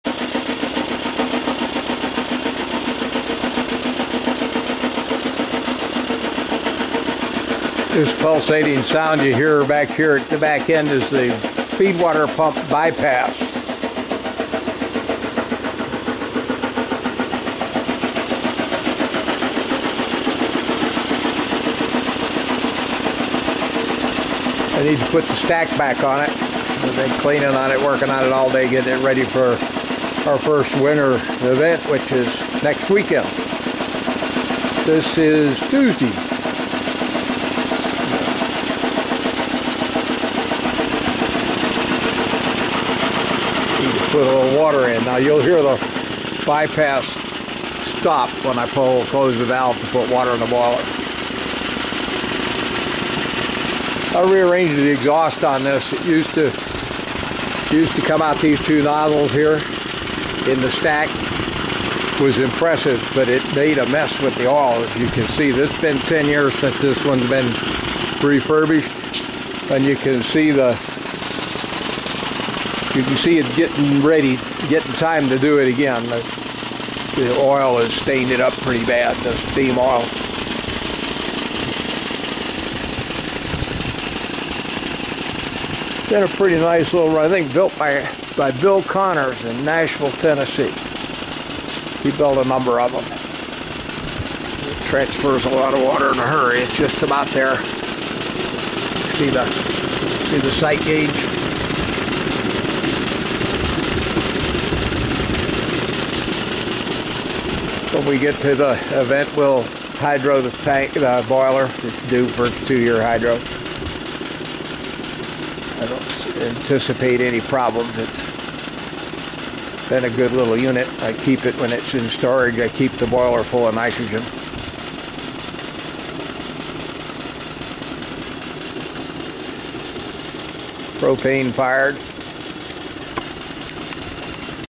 Connors Beam Engin.MPG
Connors Beam Engine running on test stand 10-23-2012